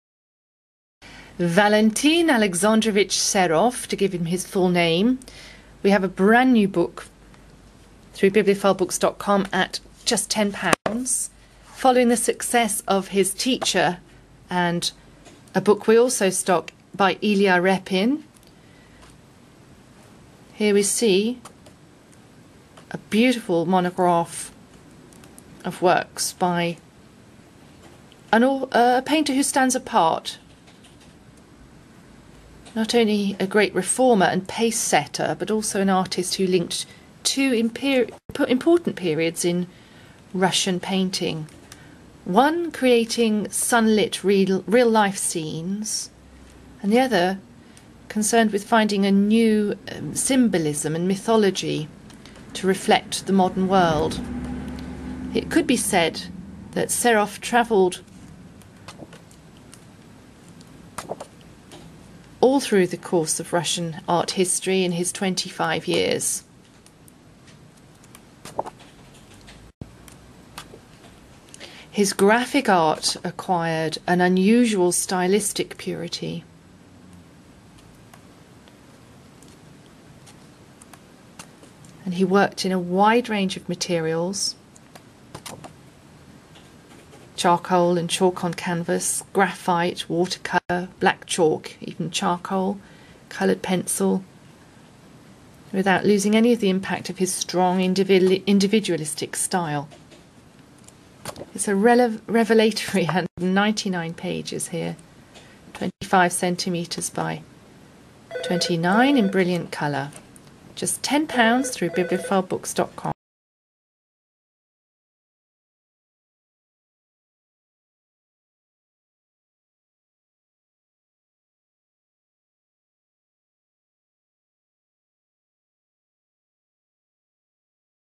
VALENTIN SEROV
DMITRI SARABIANOV    Book Number: 70243    Product format: Hardback

Valentin Alexandrovich Serov, who stands apart from other painters of the late 19th and early 20th centuries, was not only a great reformer and pace-setter but also an artist who linked two important periods in Russian painting - one creating sun-lit, real-life scenes, the other concerned with finding a new symbolism and a new mythology to reflect the modern world. It could be said that the road travelled by Russian art in the course of 25 years, bridging the turn of the century, was that from Serov's Girl with Peaches (page 16 of this volume) to his Portrait of Ida Lvovna Rubinstein (pages 116-117 idem). Without violating any of the precepts of his teacher, the famous Ilya Repin, the young artist initiated a new method which was to evolve even further in the work of most of the artists of his generation. On a par with Serov's later paintings, his graphic art acquired an unusual stylistic purity, which can be experienced in the last section of this lovingly detailed volume. He worked in a wide range of materials, pencil on paper mounted on cardboard, charcoal and chalk on canvas, graphite, watercolour and white chalk, black chalk - even charcoal, chalk and coloured pencil on yellowish paper - without losing any of the impact of his strong individualistic style. A revelatory199 pages 25cm x 29cm in brilliant colour and b/w, including archive photos of family and friends. New publication.